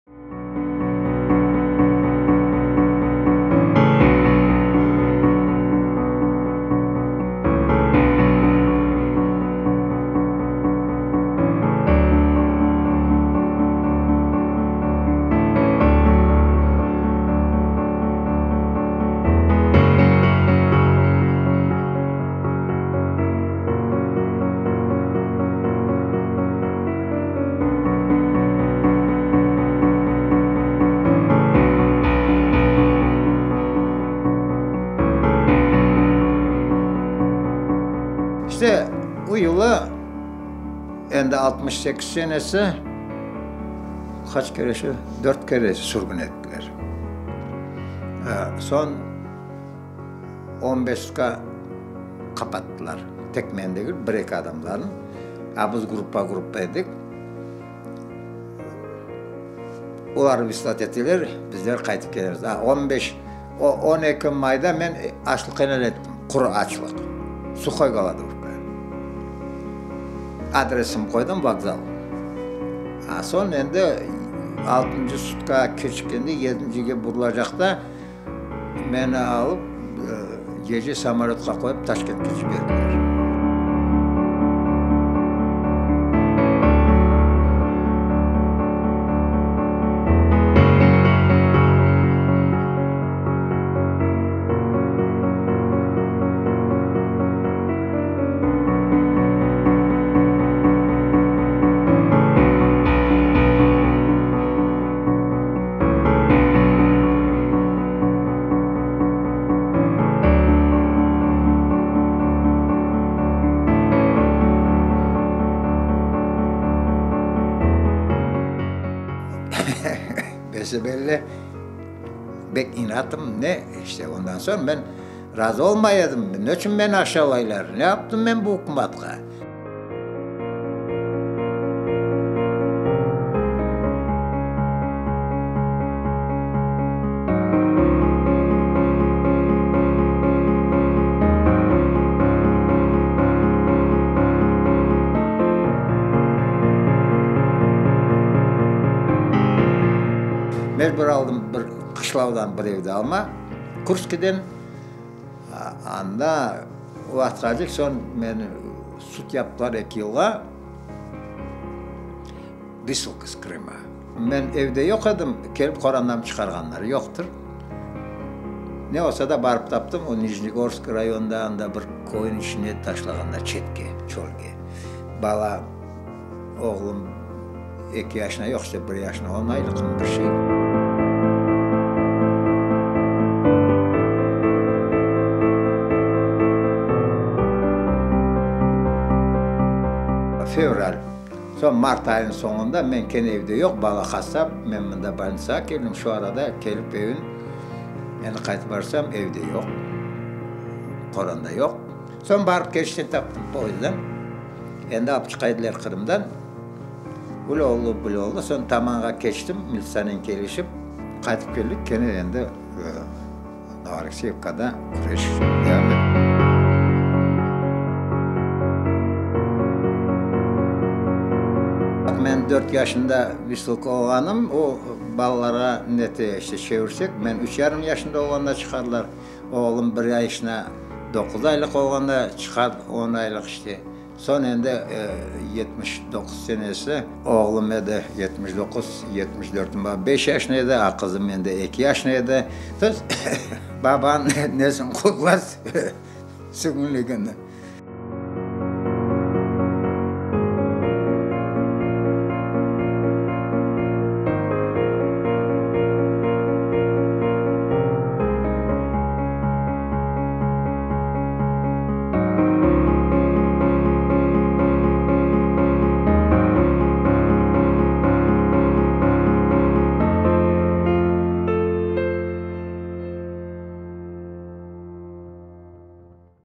Энвера Аметова выселяли из Крыма 7 раз
Ему было 4 года, когда его и весь народ обвинили в «предательстве» .Эшелоны с телами умерших родных и  соотечественников, голод и дубинки, презрение и слёзы,  боль и раны народа укрепили дух и целеустремленность Энвера Аметова.  А за годы упорного  стремления и борьбы за право жить на  Родине, он подвергался выселению семь раз.